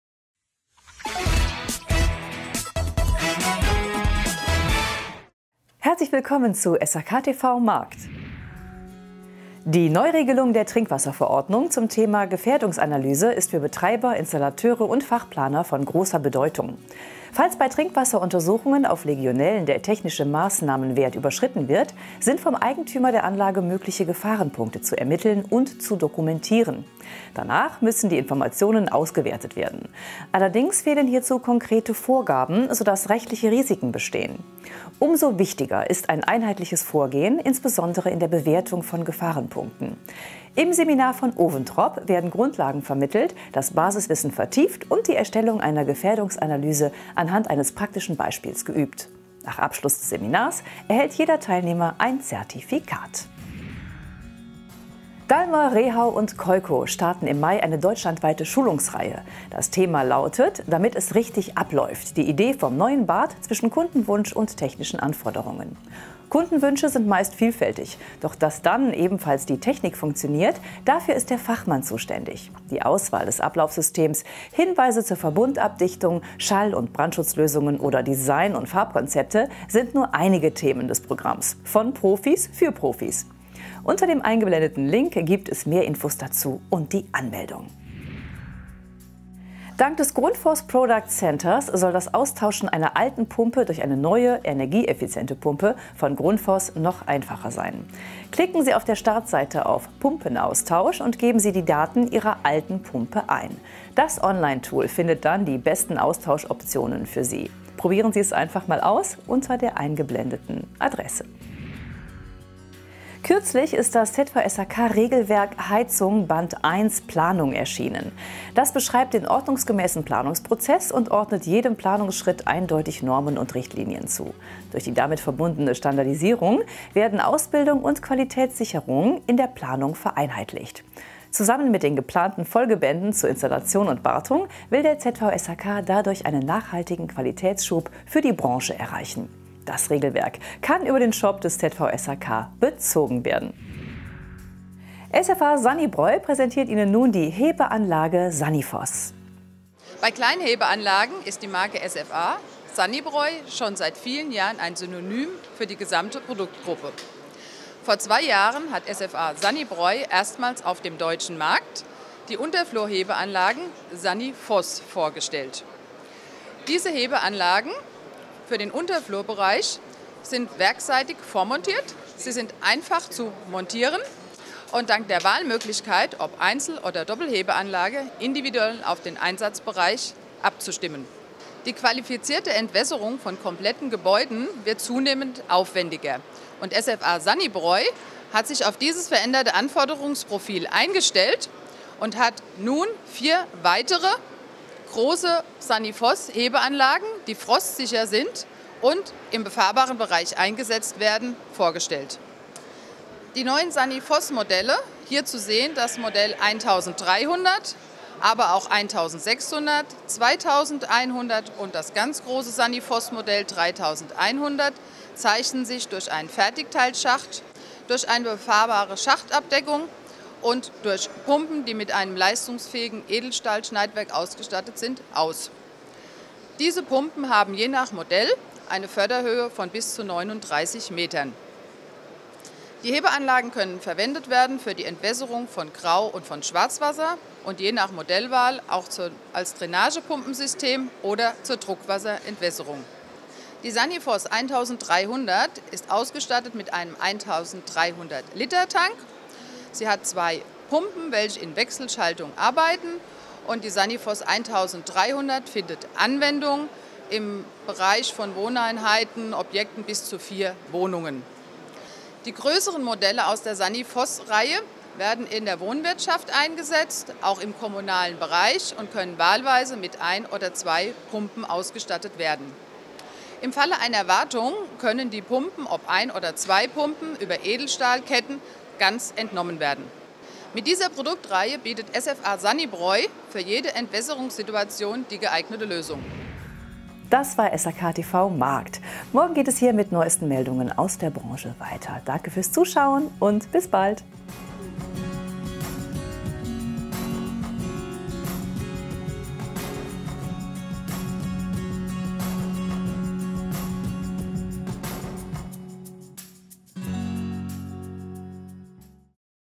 SHK-TV Markt: Gefährdungsanalyse in Trinkwassersystemen
... und mit:
– Dallmer, Rehau und Keuco: Deutschlandweite Schulungsreihe
– Grundfos: Pumpenaustausch leichtgemacht
– ZVSHK: „Regelwerk Heizung Band 1: Planung“ erschienen
– SFA Sanibroy: SANIFOS Hebeanlagen